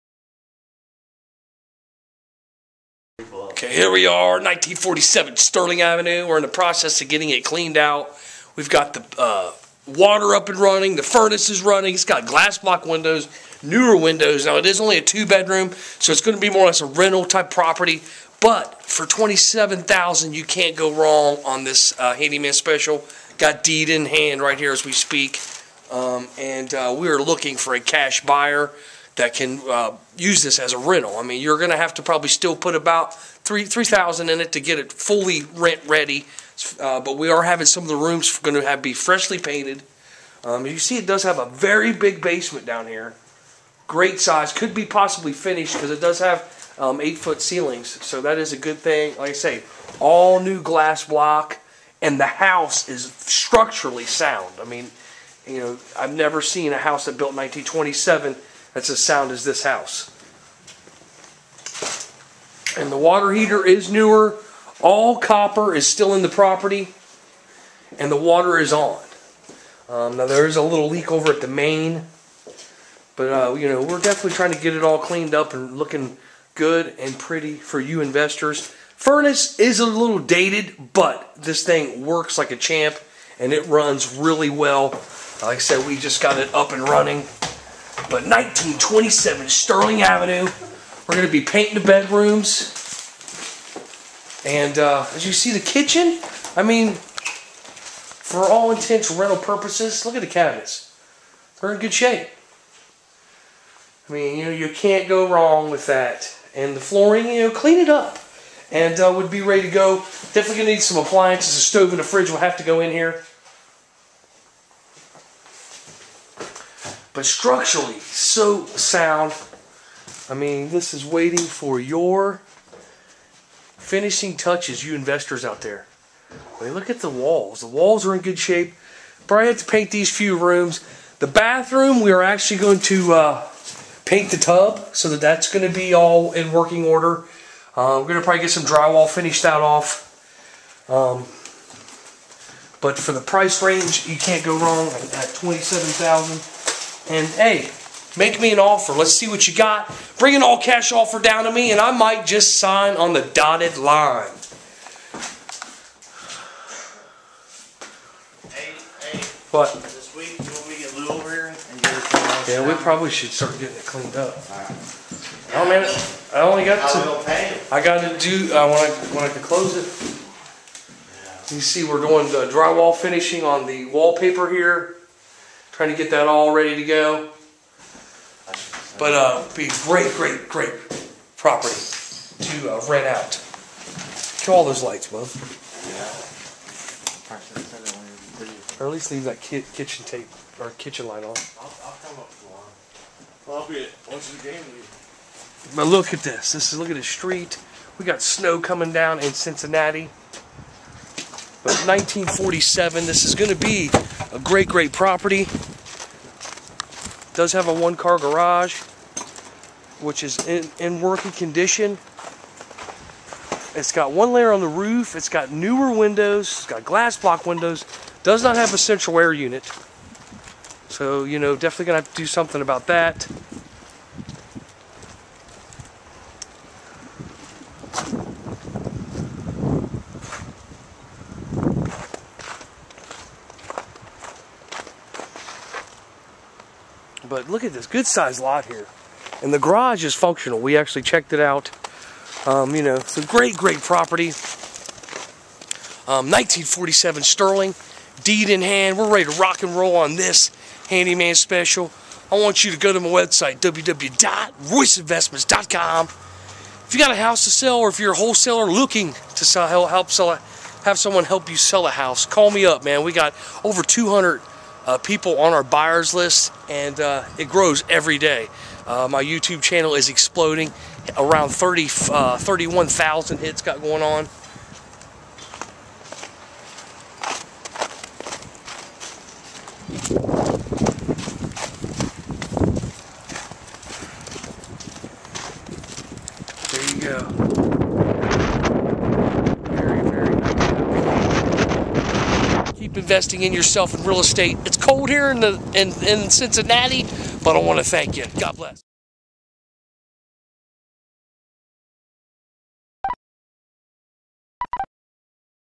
Mr. Wholesaler of Cincinnati Sending Out A Wholesaling S.O.S
This is a very pretty house in a great rental location. The home has working furnace, water is on and we are painting the bedrooms. The house only needs some minor TLC and could be a great section 8 rental